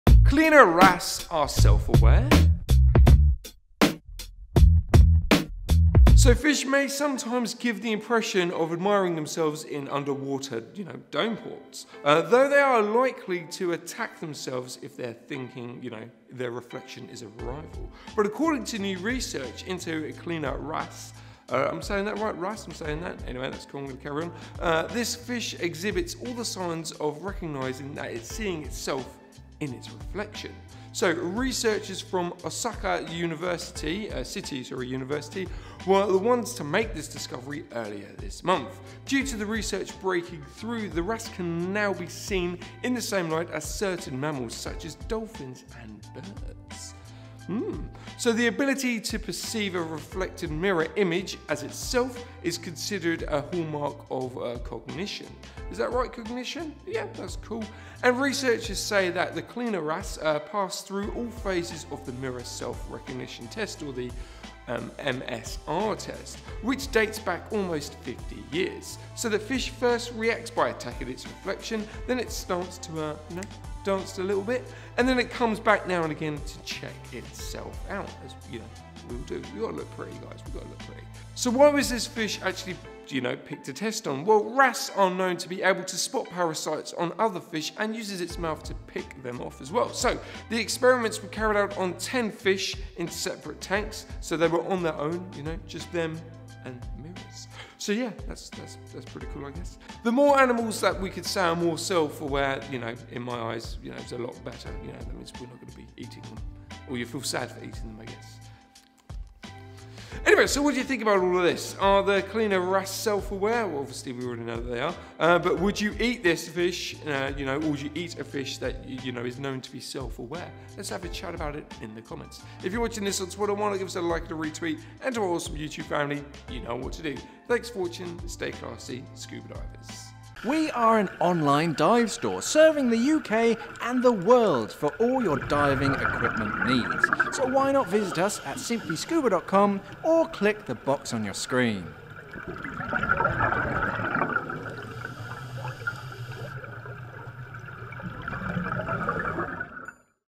Daily Scuba News - Cleaner Wrasse Are Self-Aware
Cleaner Wrasse Are Self Aware

 In today's Daily Scuba News Shaun is going to talk about the Cleaner Wrasse and how researchers and scientists have found out that they are in fact self-aware... which is pretty cool.

::Contributors::
- Divernet
- Pixabay
- Shaun Johnson
- Mark Newman